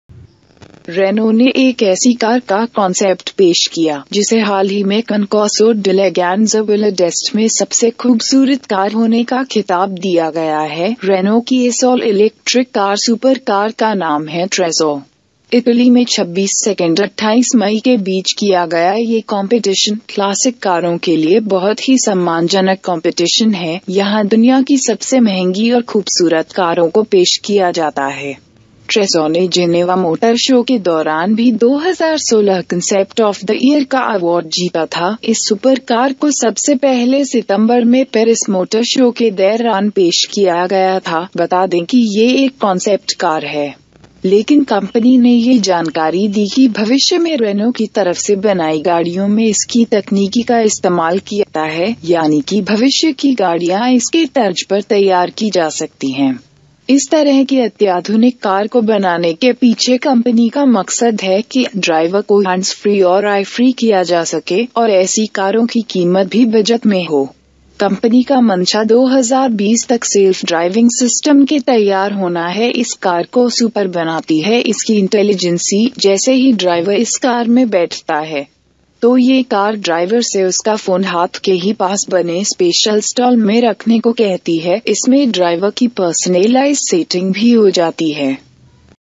Renault's electric supercar was named the most beautiful concept car of the year
Renault's all-electric supercar, dubbed the Trezor , was recently voted the most beautiful concept car at Concorso d'Eleganza Villa d'Este, which is a prestigious competition for classic cars hosted annually on the shores of Lake Como in Italy.
The event, which took place May 26-May 28, is known for drawing some of the most expensive cars in the world. The Trezor won the top spot for best design in the prototype and concept category. In March, the vehicle was also named the 2016 Concept Car of the Year at the Geneva Motor Show.

The Trezor, which was originally unveiled in September at the Paris Motor Show, is purely a concept vehicle for now. However, the company said it will inspire design elements in future Renault vehicles.